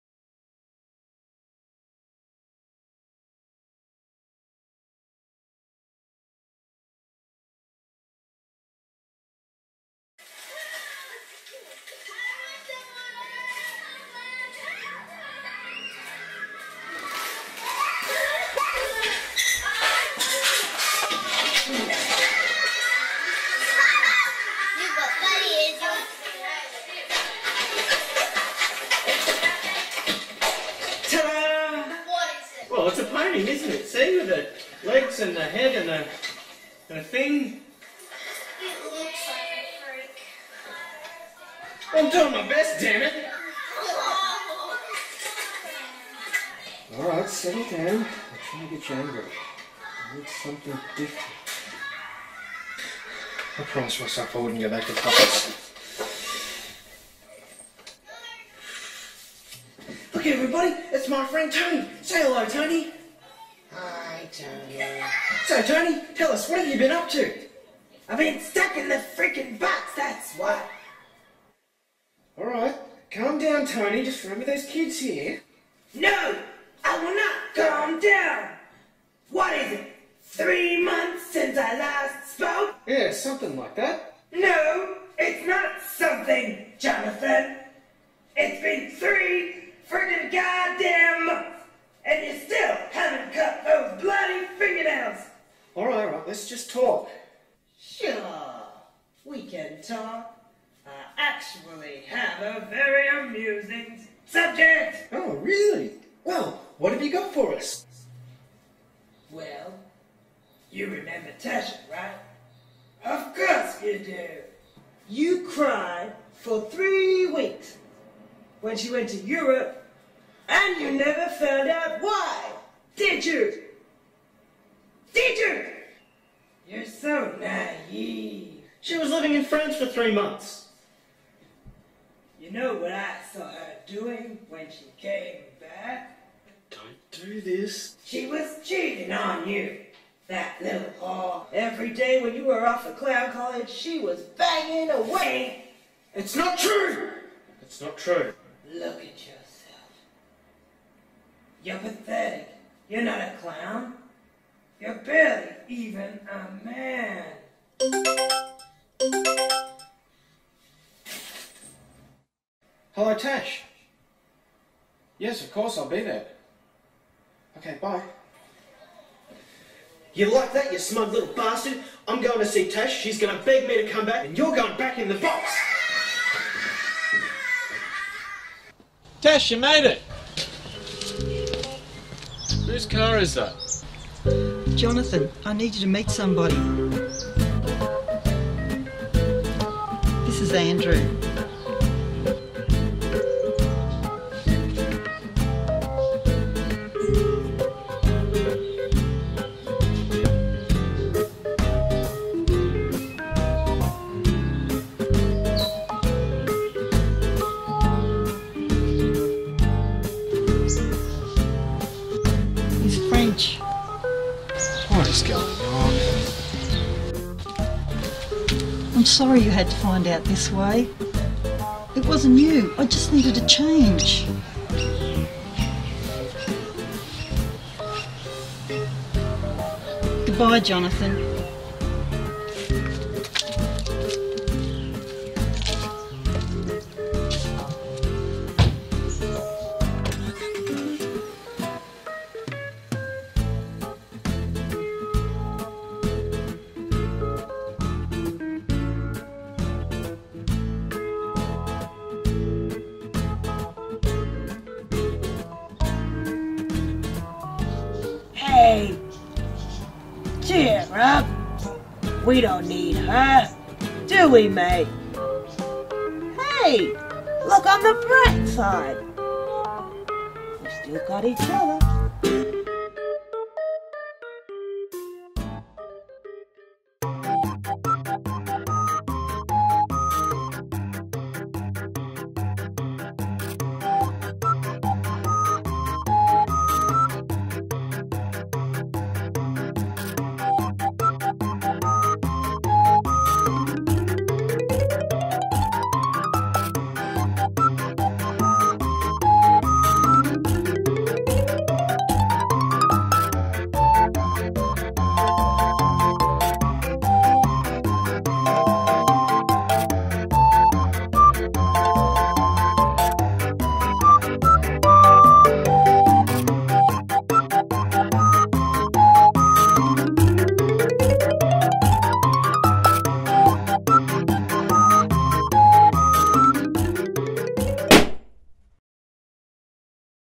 No Funny Business
A party Clown resorts to hand puppetry, with unusual outcomes